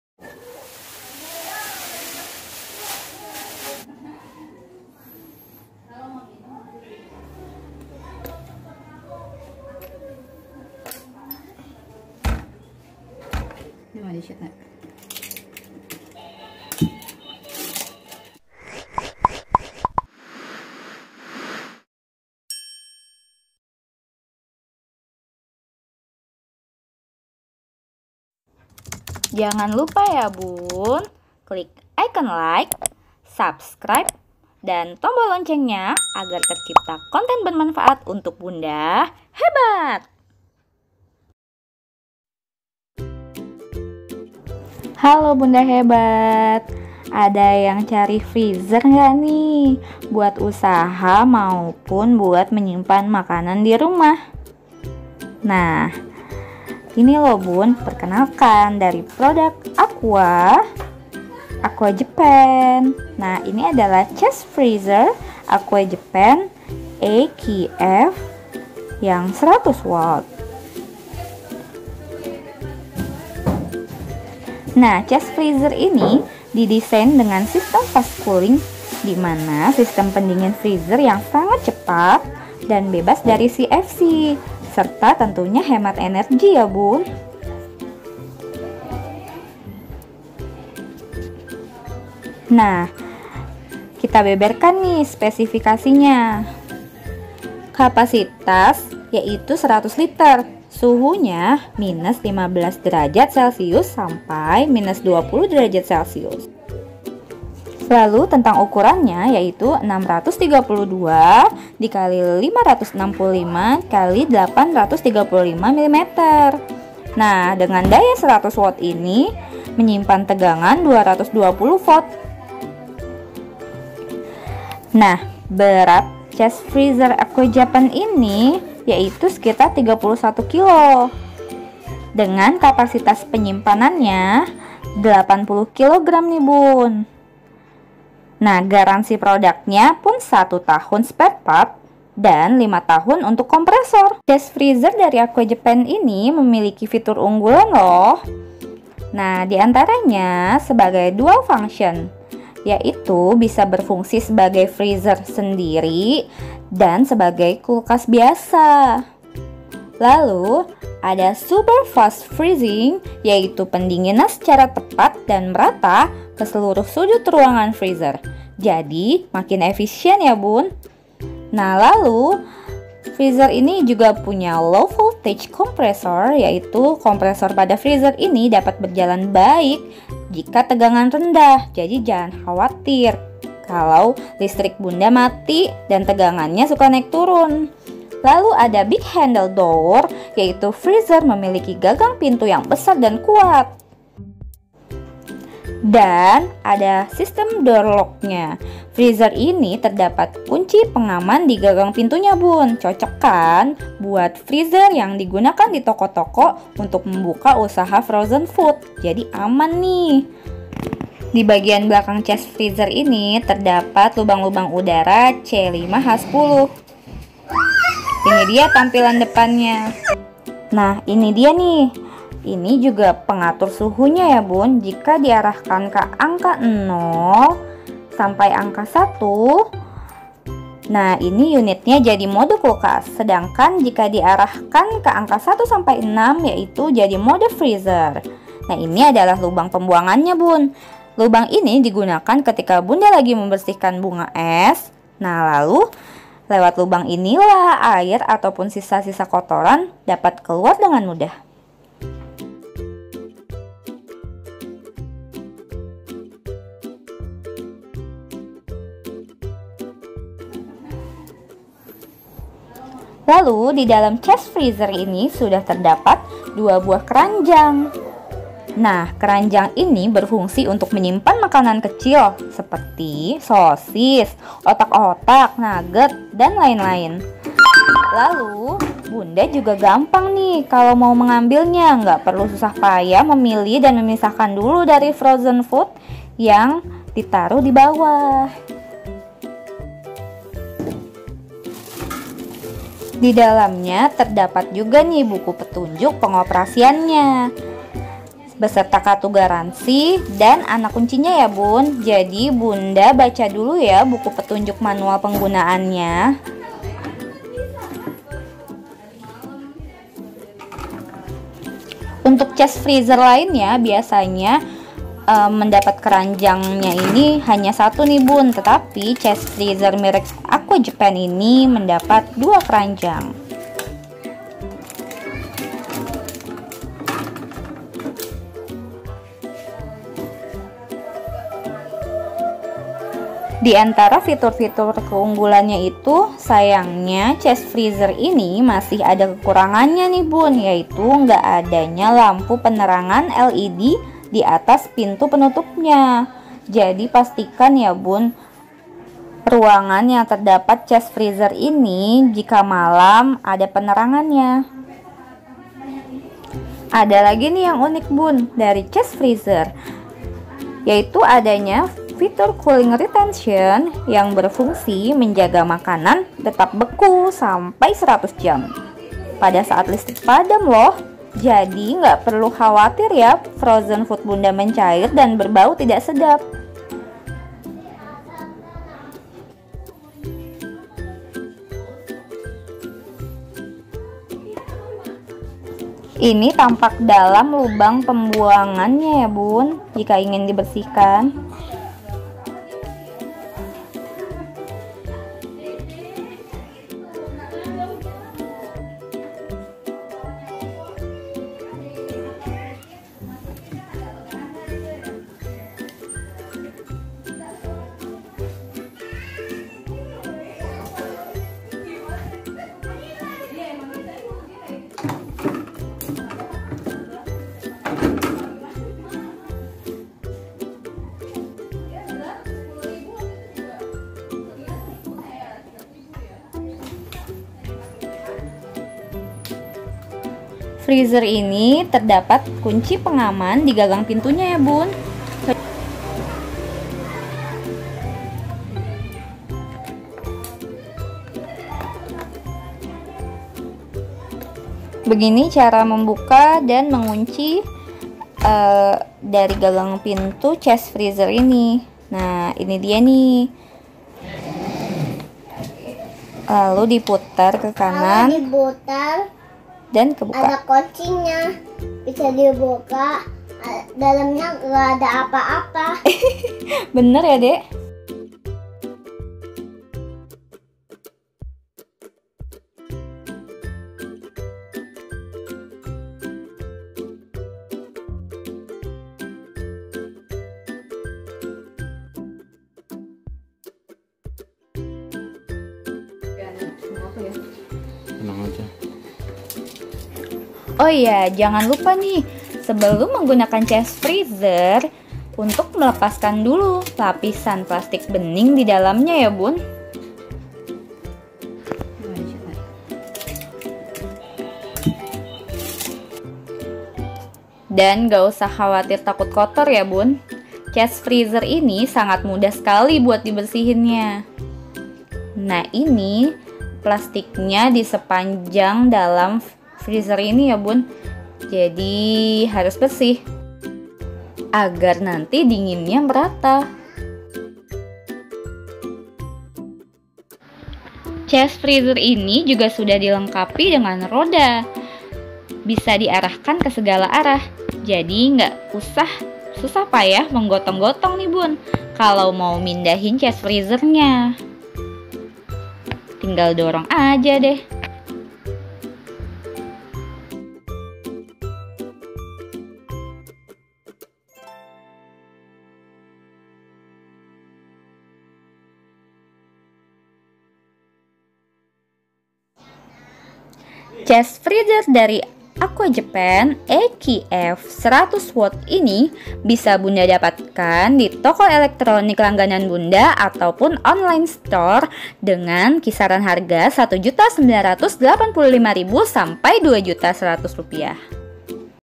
Freezer ini Cocok Banget Untuk Usaha Frozen Food Rumahan Tahan 100 Jam Setelah Listrik Mati
Freezer ini Cocok Banget Untuk Usaha Frozen Food Rumahan Tahan 100 Jam Setelah Listrik Mati | Chest Freezer Aqua Japan AQF 100 W

Aqua AQF 100 W Freezer, chest freezer yang didesain dengan
sistem fast cooling dimna sistem pendingin freezer sangat
cepat dan bebas dar CFC hemat energi

Ada yg unik nih bun dari Chest Freezer ini, yaitu adanya fitur Cooling Retention yg berfungsi menjaga makanan tetap beku sampai 100 jam saat listrik padam. Jadi nggak perlu khawatir frozen food bunda mencair dan berbau tidak sedap ya bun.